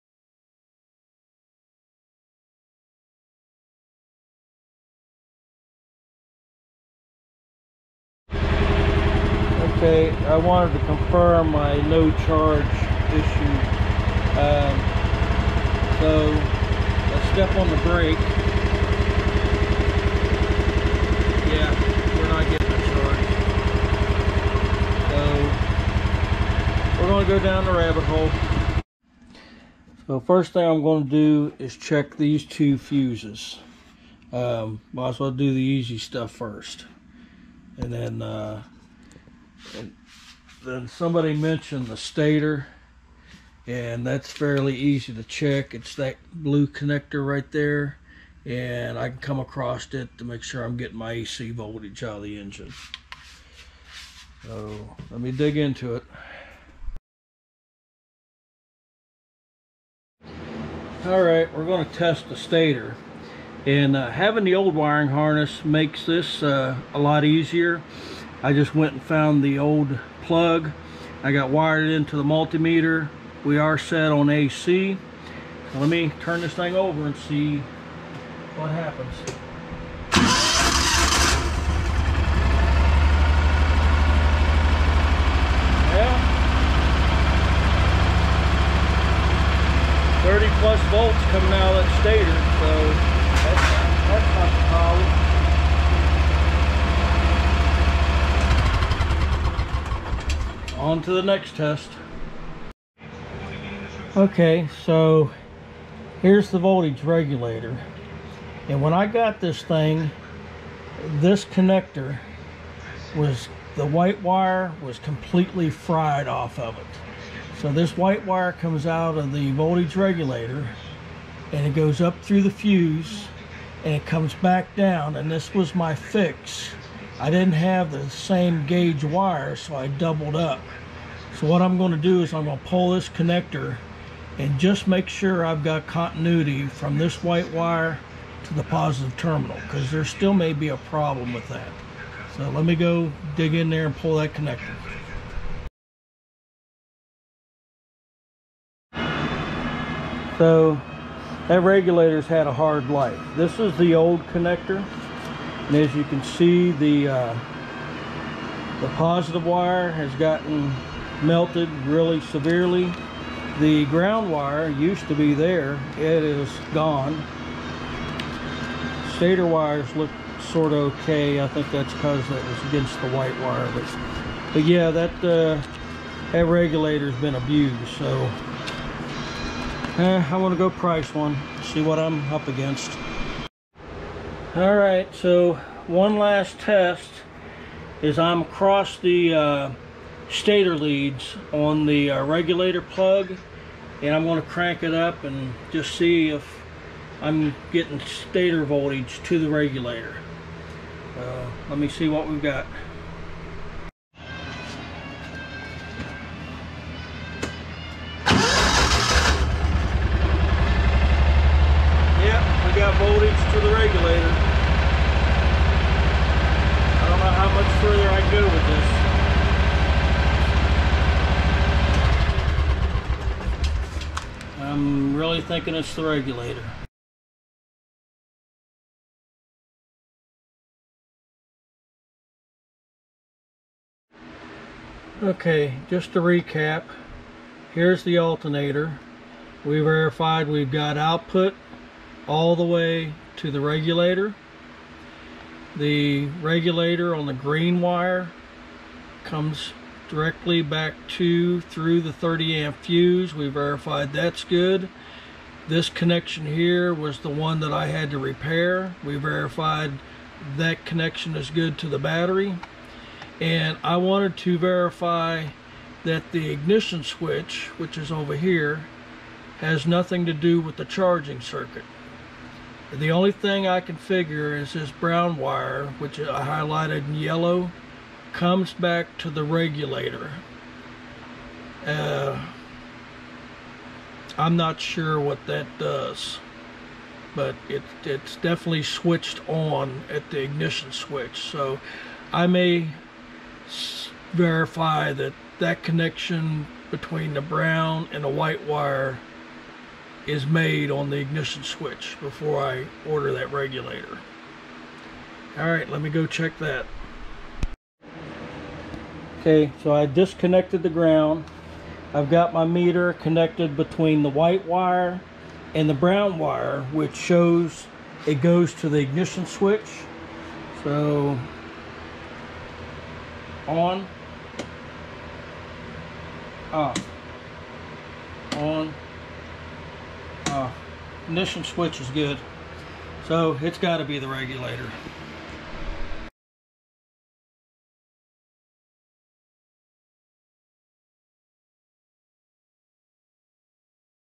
Testing the Mule voltage regulator
The Mule was getting hard to start, and I found the battery wasn't charging. I wanted to make sure it was the regulator before ordering a new one.